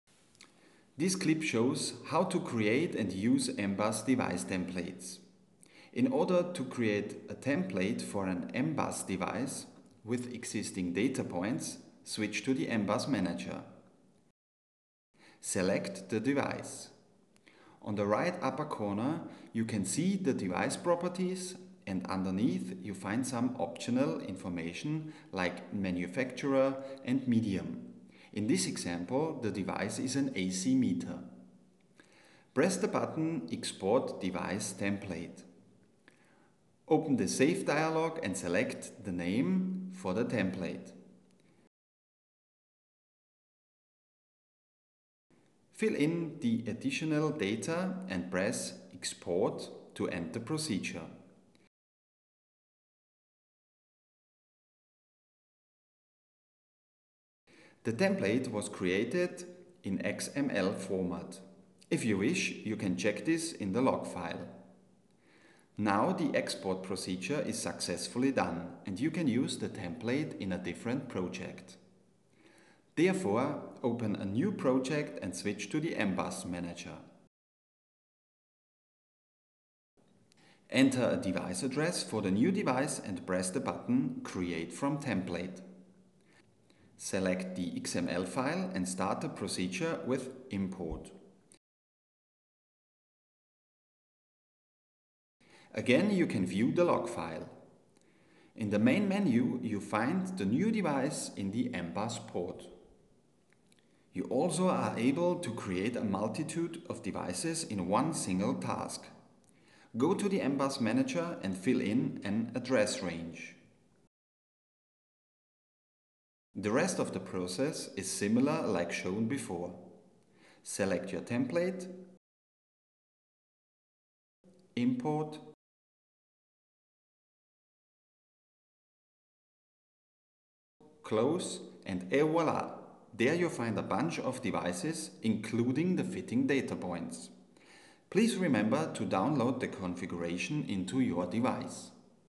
LOYTEC L-INX Configurator - How to create and use M-Bus device templates [L002]
Demonstration how to create and use the M-Bus Device Templates using a LINX-100 Automation Server.